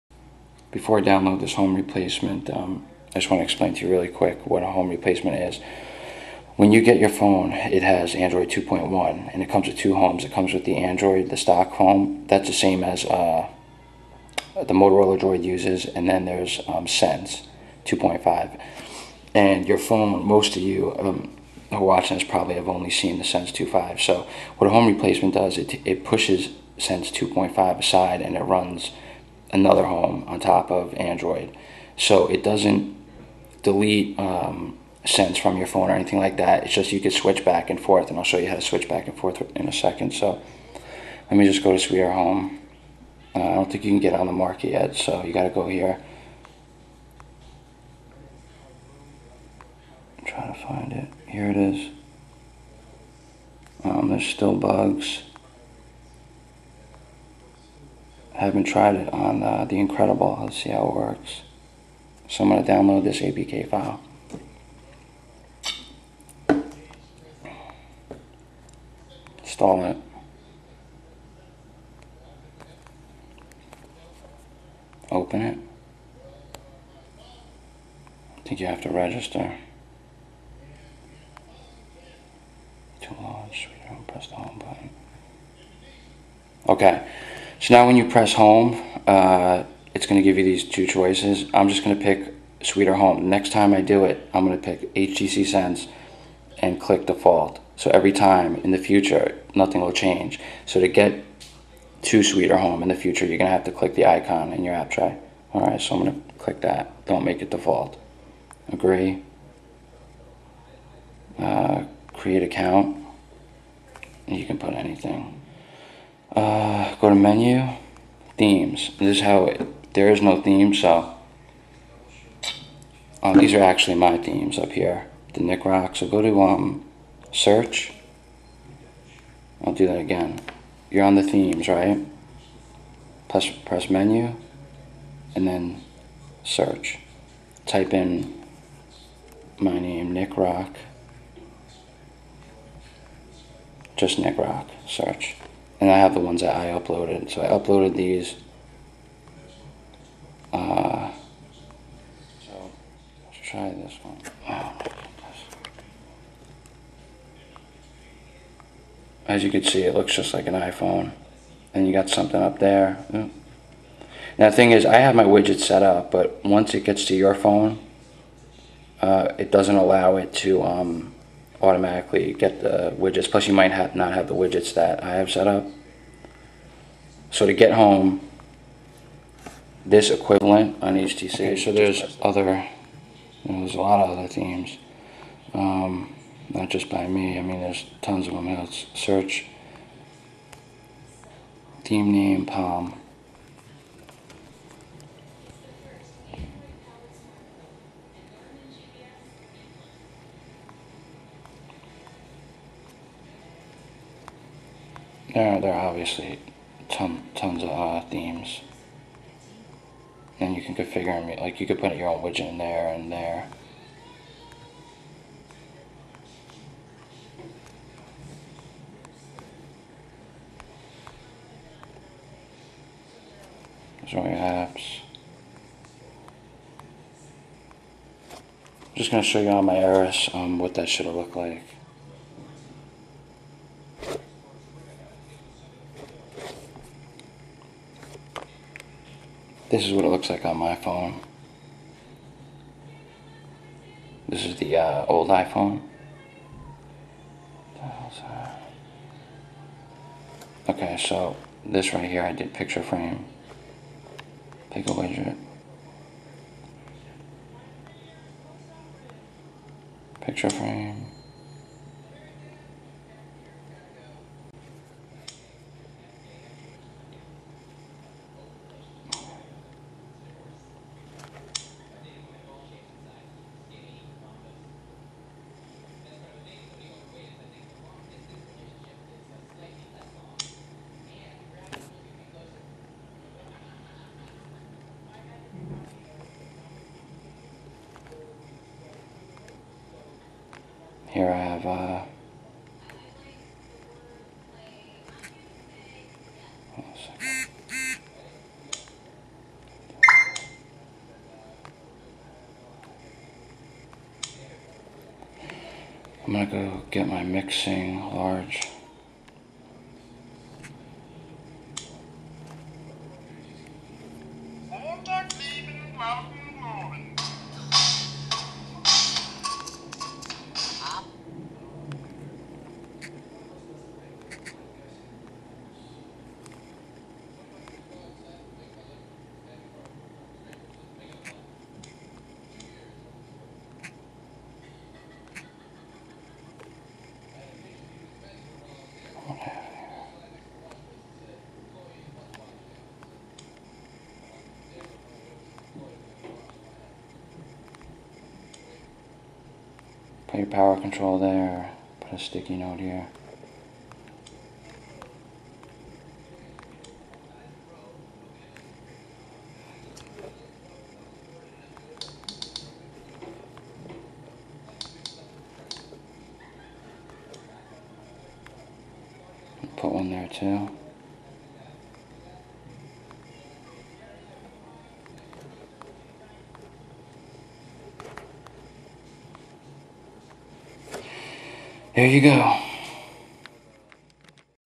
HTC Incredible - Apps & Widgets - Advanced
Explanation and demo of my favorite Home Replacement at the moment.